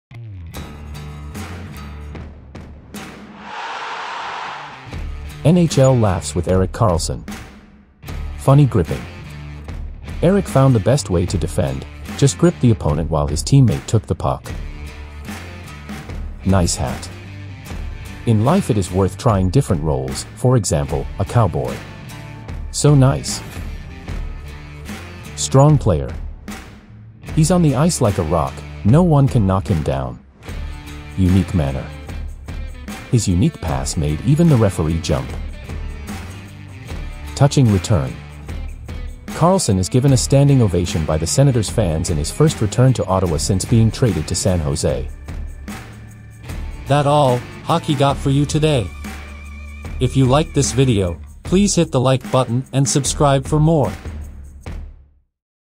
NHL laughs with Erik Karlsson😆😀😂
🏒🏒🏒Make you day with Erik Karlsson funny moments😂😂😂

Erik Karlsson is the clear front-runner and he's the league’s best rearguard. Above all that, he adores entertaining people around him. See yourself!